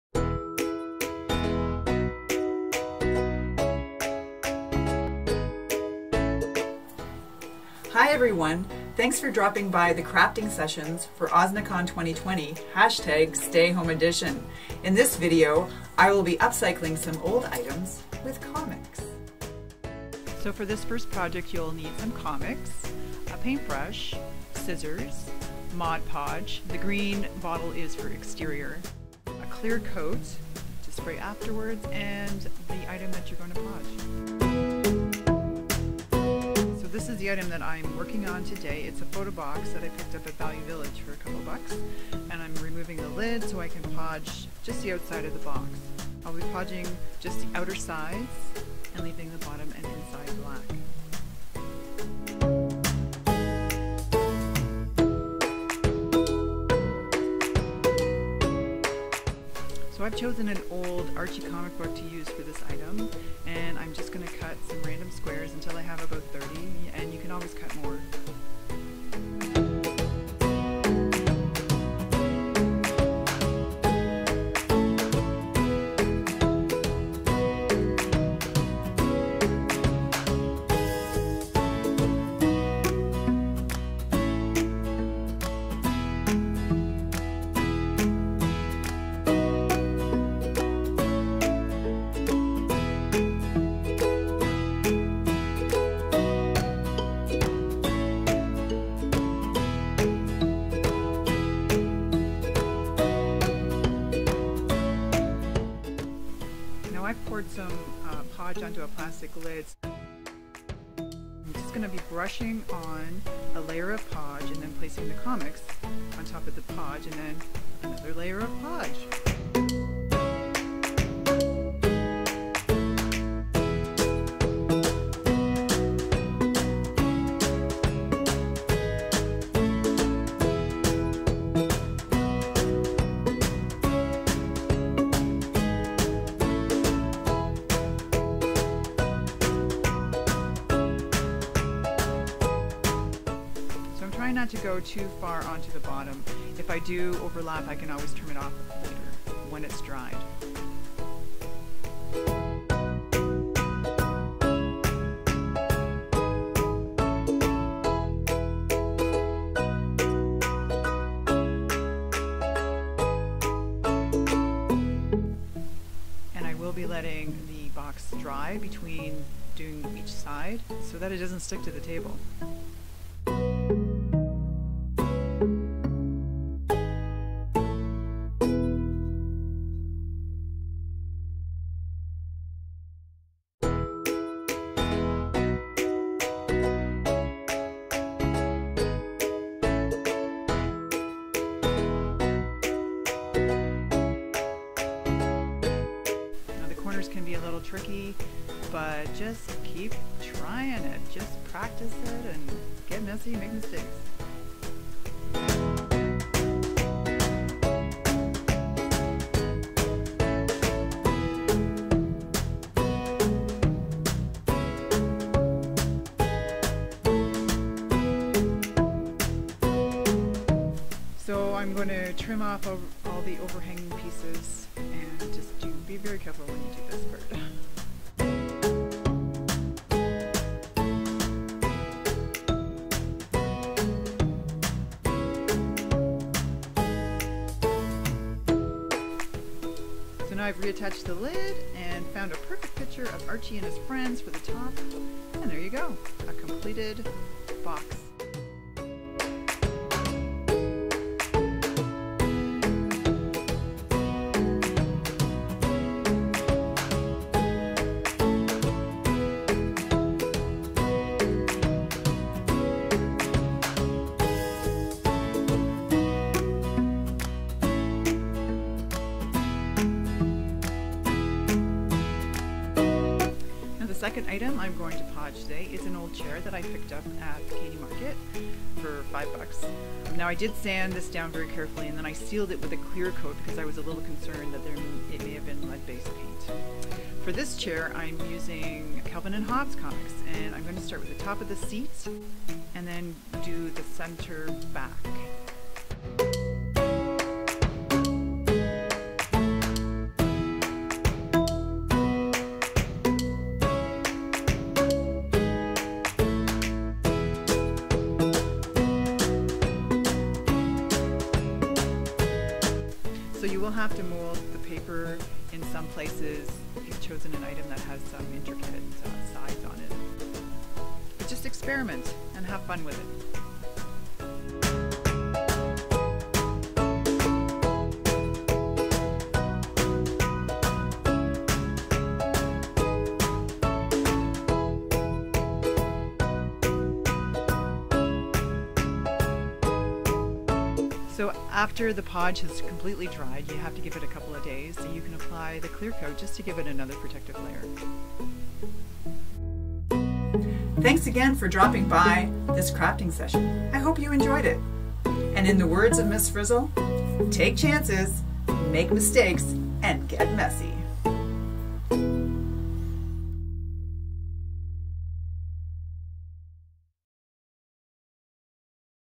Up-cycling with Comics - OSMiniCon 2020
Welcome to Crafting with Sharon: Up-cycling with Comics, part of the OSMiniCon2020 stayhome edition which ran on May 2, 2020.

Get a few tips on how to breathe new life – and colour! - into everyday items with a little bit of podge and some old comics.

Please share your finished upcycled items with us on social media! Tag with OSMiCon2020